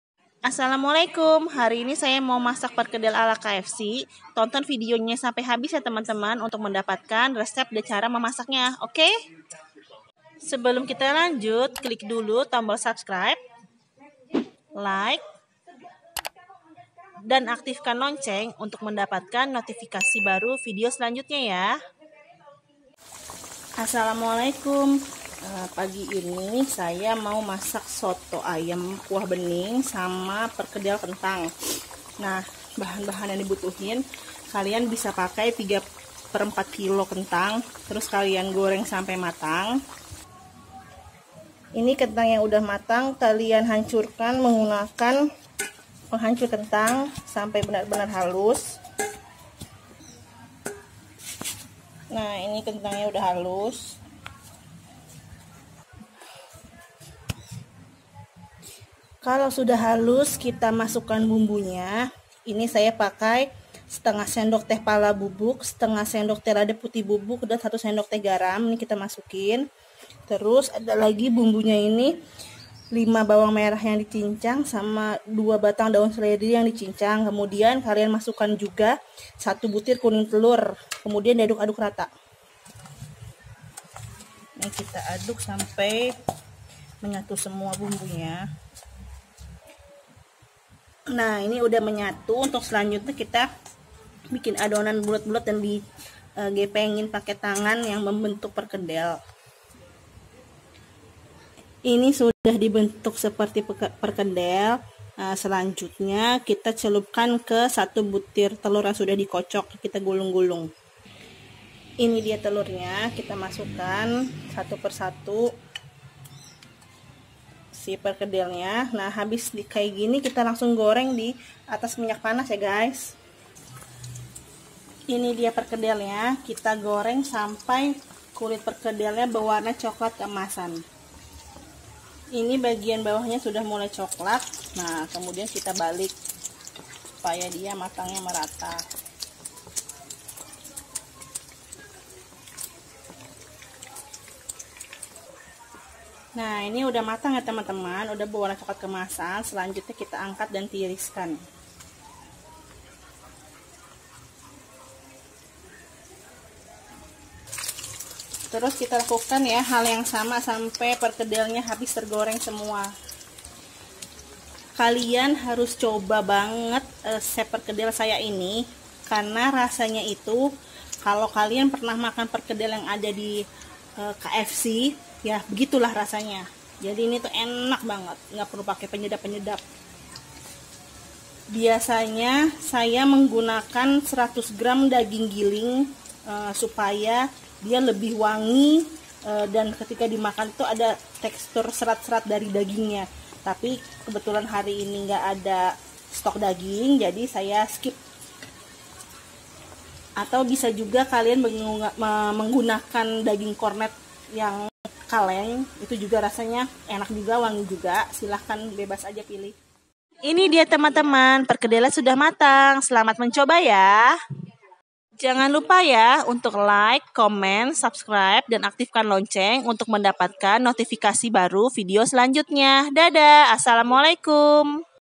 Resep perkedel kentang ala KFC
Bahan :

- 3/4 kg kentang
- 1/2 sdt pala bubuk
- 1/2 sdt lada hitam bubuk
- 1 sdt garam
- 5 bawang merah (cincang)
- 2 tangkai daun seledri (cincang
- 1 kuning telur
- 100 gram daging giling